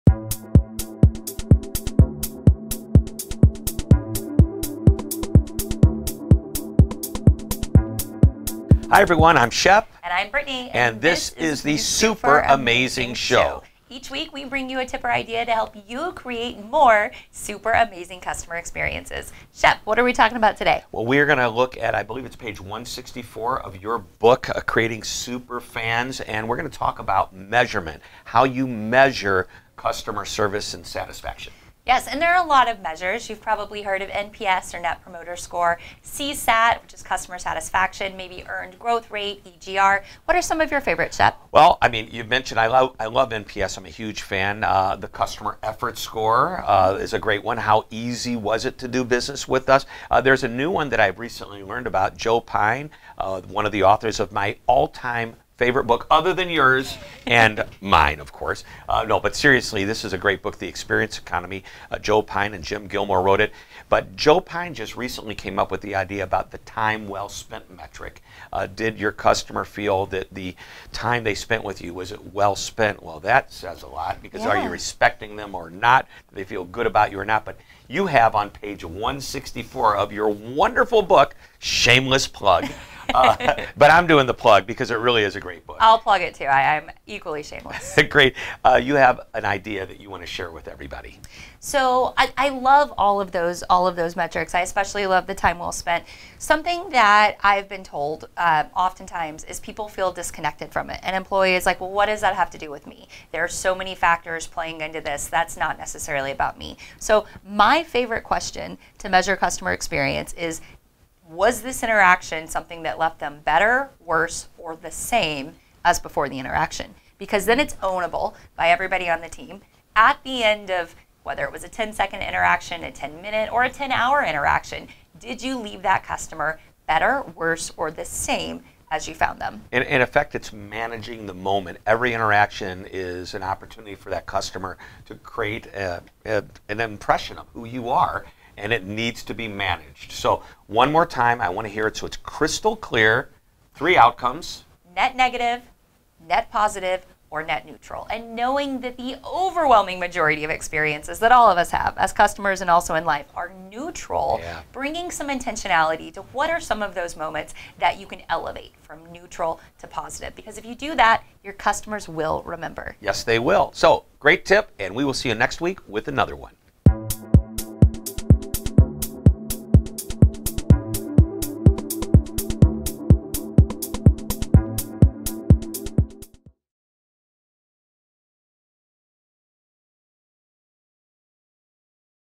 A Simple Way to Measure CX
How do you measure customer experience?

Personally, my favorite way to measure CX is this simple framework:

For every one-to-one interaction you have, there are three possible outcomes for the other person. At the end of the exchange, they’ll feel better, worse, or the same as they did before.

Here’s a secret to not only great customer experience, but also to making your entire life better: try to make as many interactions as possible net positive experiences.